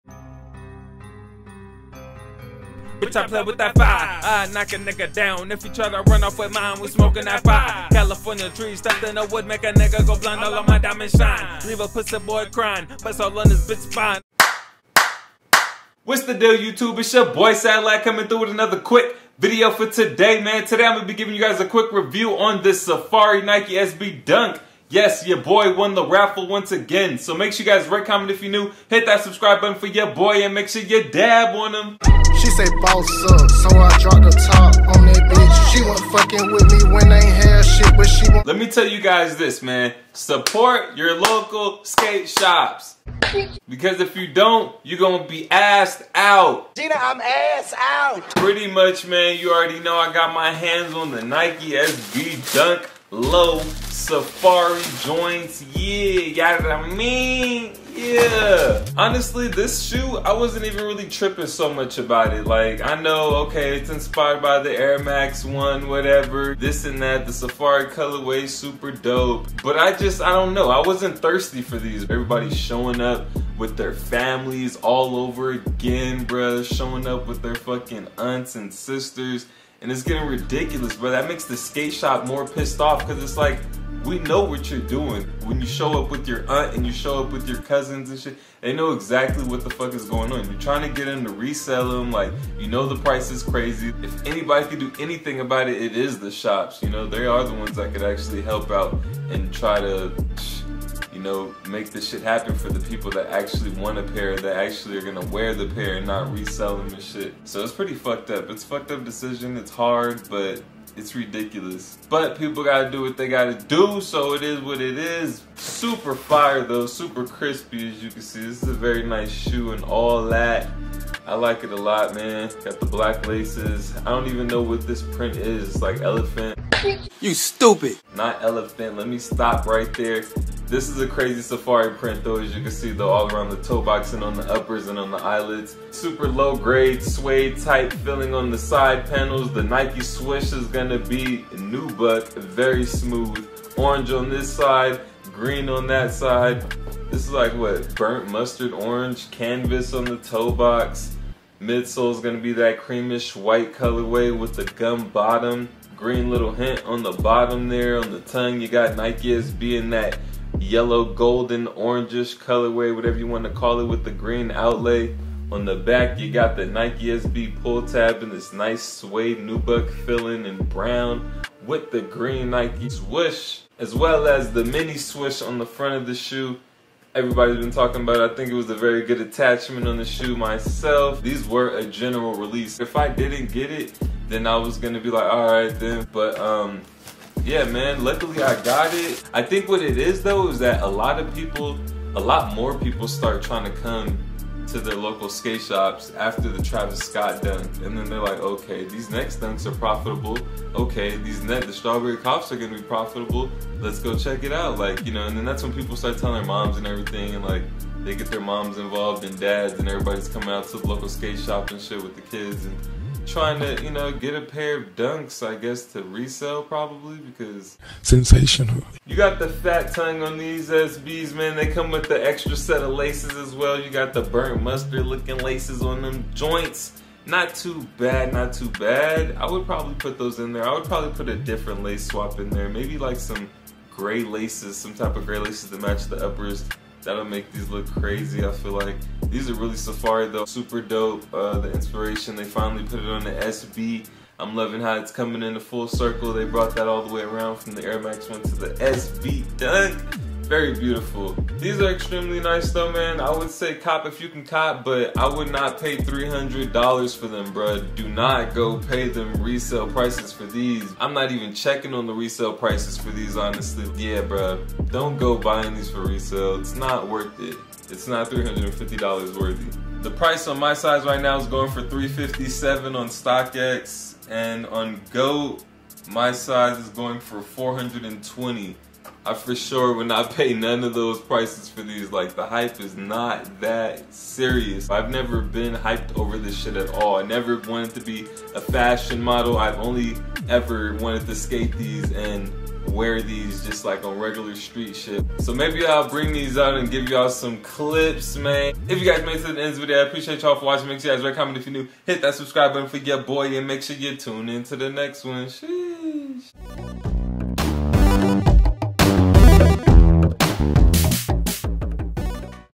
Nike Sb Dunk Low Safari Review!
What’s the deal YouTube it’s ya boy sadd coming back with another quick unboxing video review for the day! Today I got the call and was notified to pick up my Nike Sb dunk Safari lows at my nearest skate shop and no cap I’m not even all that excited.